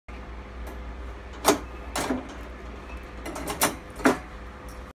Locking Bottom Cage With Second Lock
Shows how to properly place the second lock so it secures the bottom panel with the door.